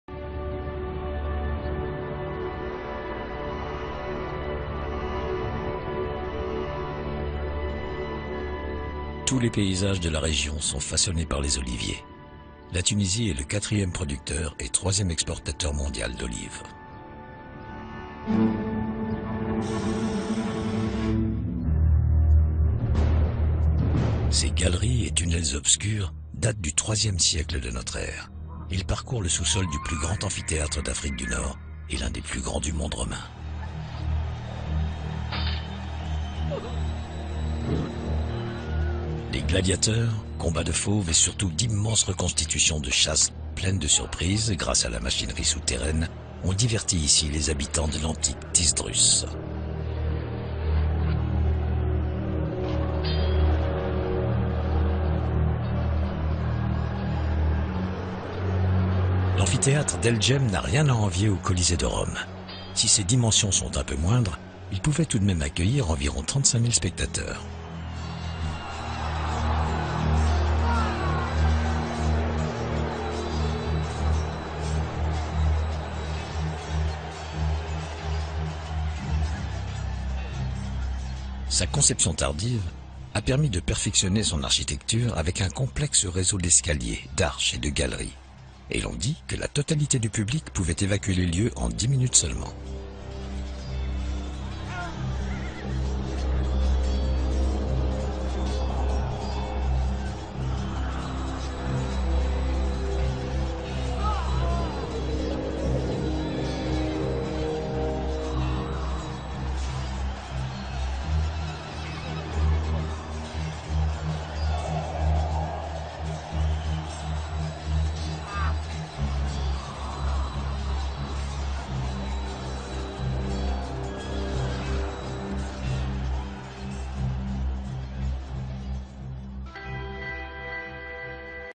The Amphitheatres of El Jem - The Roman Empire
Dans la petite bourgade d'El Jem s'élèvent les ruines impressionnantes du plus grand colisée d'Afrique du Nord, immense amphithéâtre où pouvaient prendre place 35 000 spectateurs. Cette construction du IIIe siècle illustre l'extension et la grandeur de l'Empire romain.